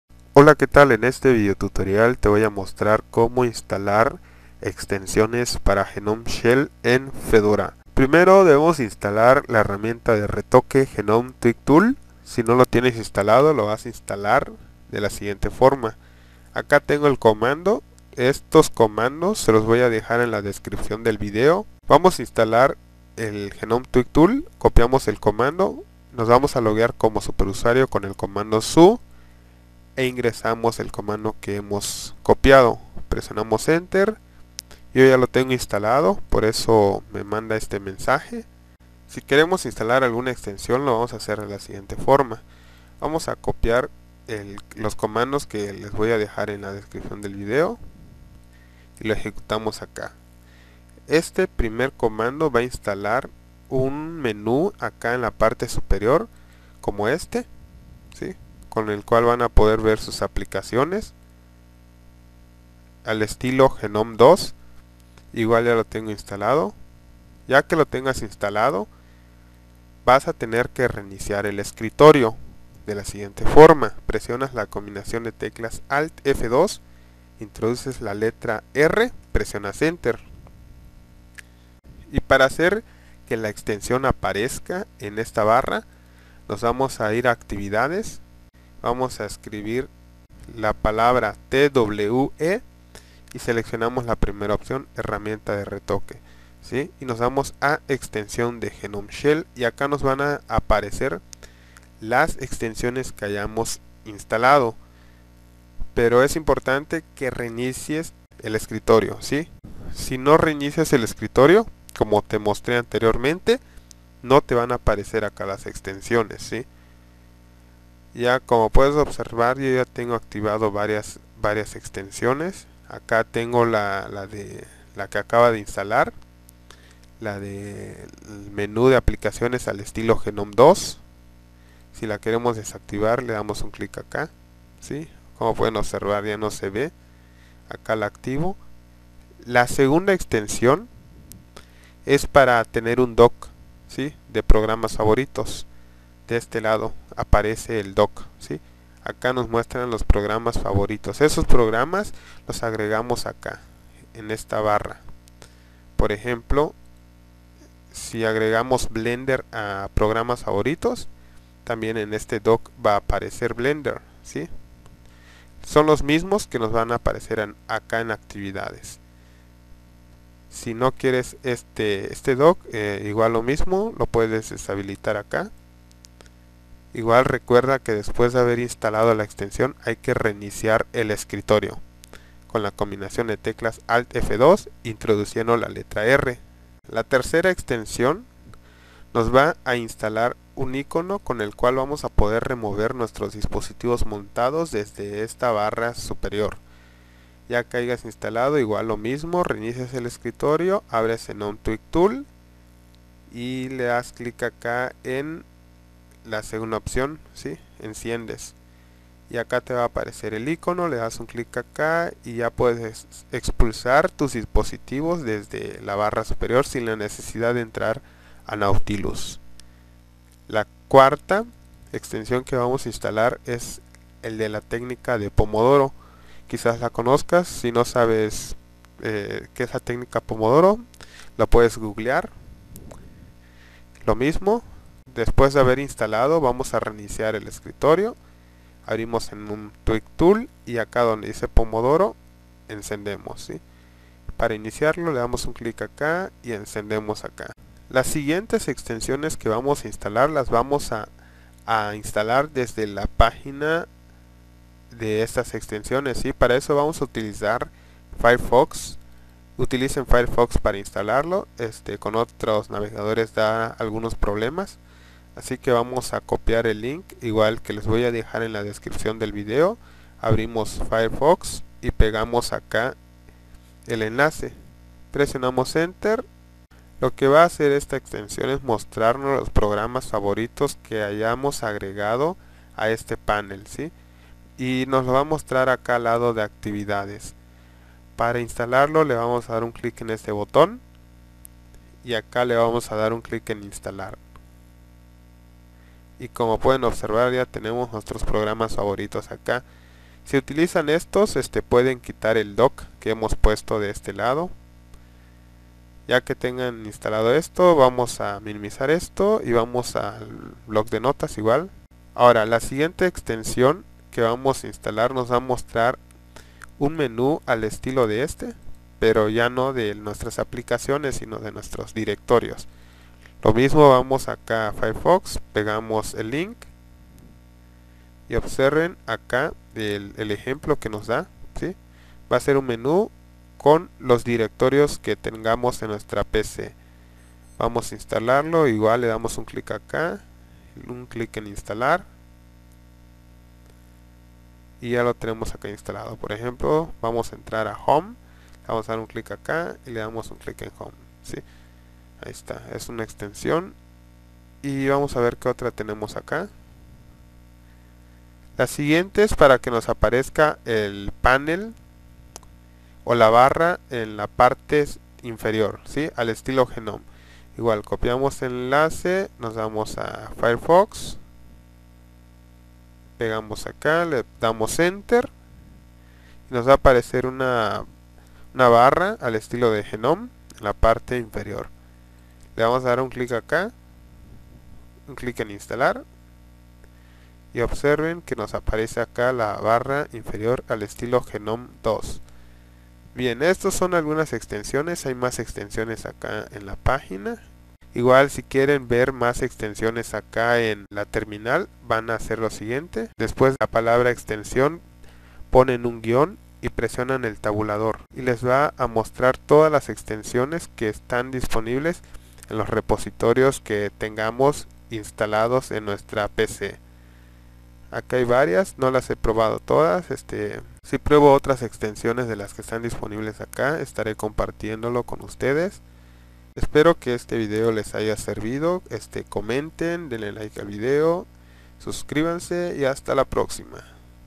Extensiones Gnome-Shell 3.6
Instalar extensiones Gnome-Shell 3.6

Aquí también estan:

Primero vamos a instalar la herramienta de retoque "Gnome-Tweak-Tool":
yum -y install gnome-tweak-tool

Extensión para un menu estilo Gnome 2 para las aplicaciones.
yum -y install gnome-shell-extension-apps-menu

Extensión de un dock de programas favoritos.
yum -y install gnome-shell-extension-dock

Extension de los discos montados, para expulsarlos desde la barra superior de Gnome.
yum -y install gnome-shell-extension-drive-menu

Extensión de la técnica pomodoro.
yum -y install gnome-shell-extension-pomodoro

Iconos alado del boton de actividades.

Menú de directorios al estilo de la extensión del menú de aplicaciones que anteriormente se mencionó en este post.

Barra en la parte inferior, el panel inferior que te ofrecía GNOME 2.